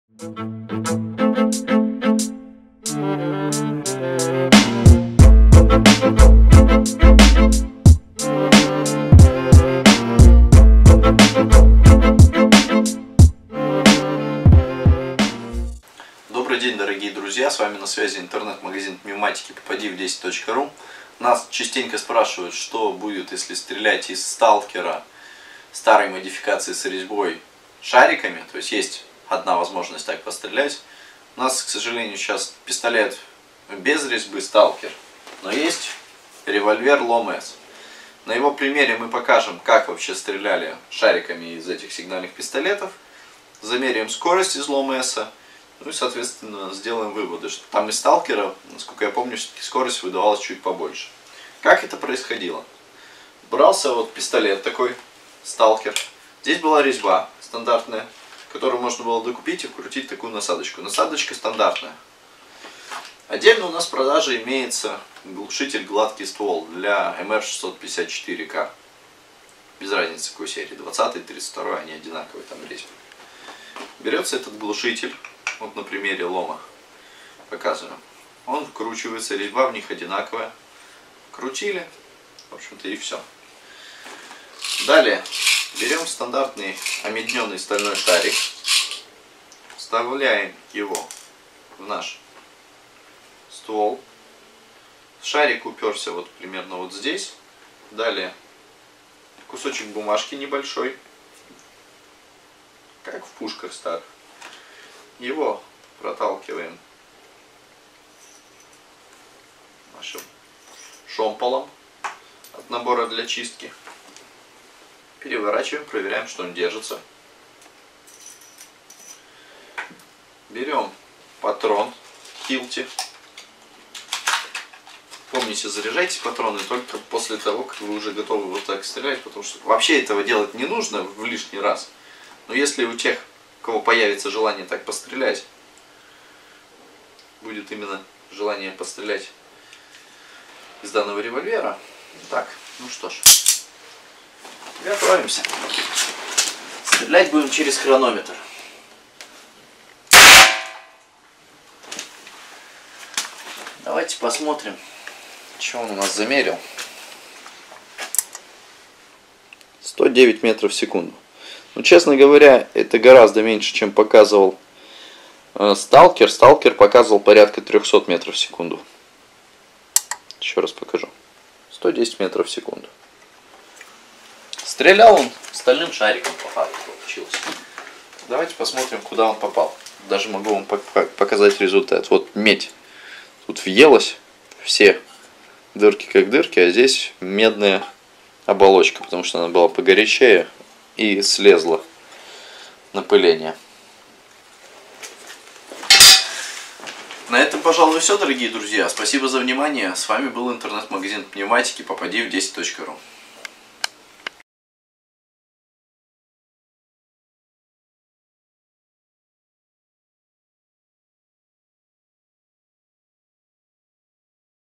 Выстрел шариком с сигнального пистолета LOM S Купить popadiv10.ru
В данном видео производим выстрел шариком с сигнального пистолета LOM-S, видео записано в качестве пробы, настоятельно не рекомендуем использовать данное оружие не по назначению и подвергать различного рода переделкам, переделки стандарта сигнального оружия преследуются по закону!